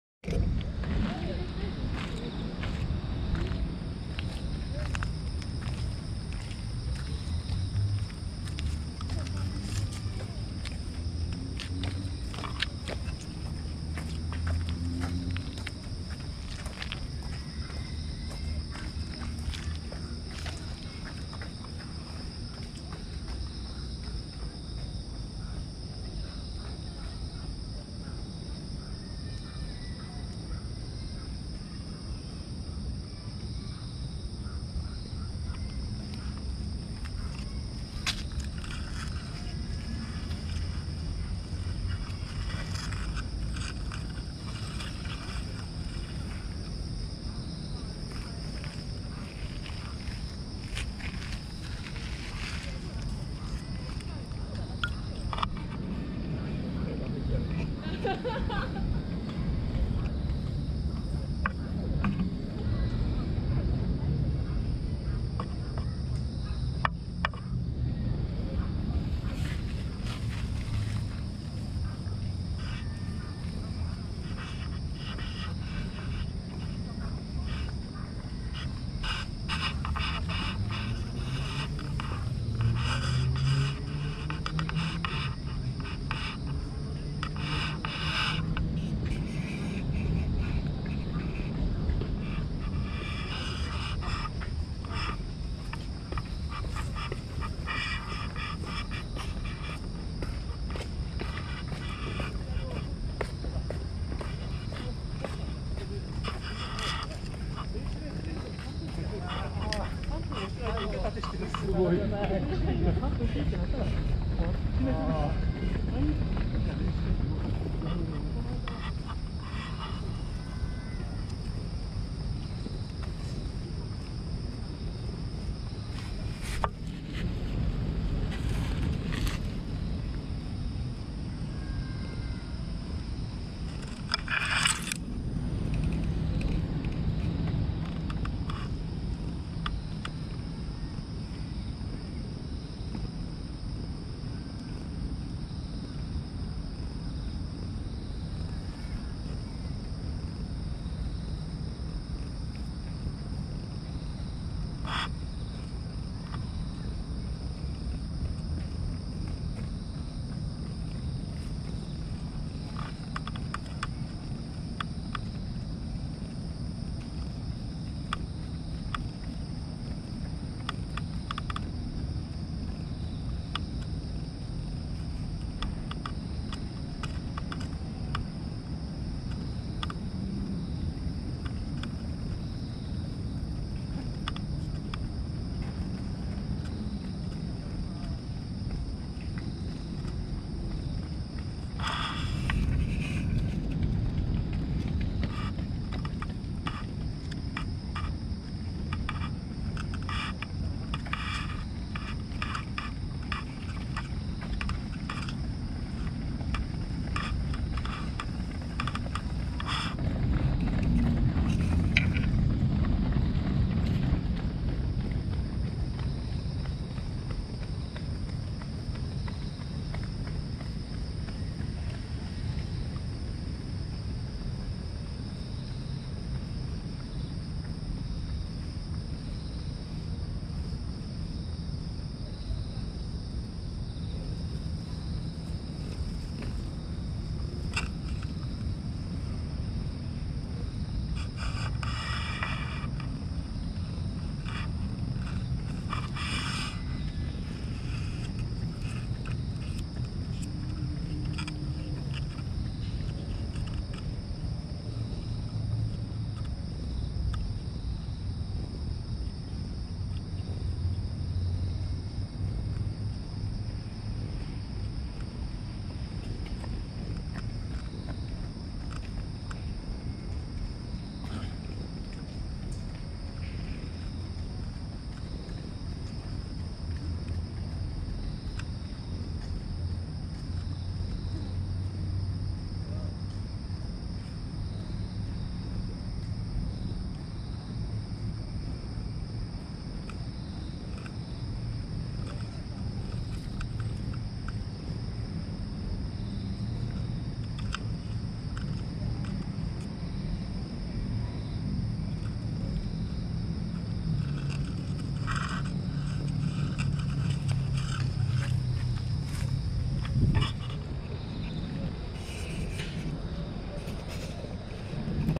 お尻トントンでごろんしてなでなでしてたらのどを鳴らして喜んでくれた可愛い野良猫
公園でずっとなでなでしていたい
可愛いキジ白の猫ちゃんを見つけました(^^♪
お尻トントンしたらごろんしてくれて、
なでなでしていたらのどを鳴らして
喜んでくれました(#^^#)
しぐさがなんか可愛くて
たまりませんでした(*^▽^*)

他にも可愛い猫ちゃんたちの動画アップしてます！

おすすめネコ動画

あの「感動猫動画」のサブチャンネルの

飼い猫と野良猫と一緒に暮らしている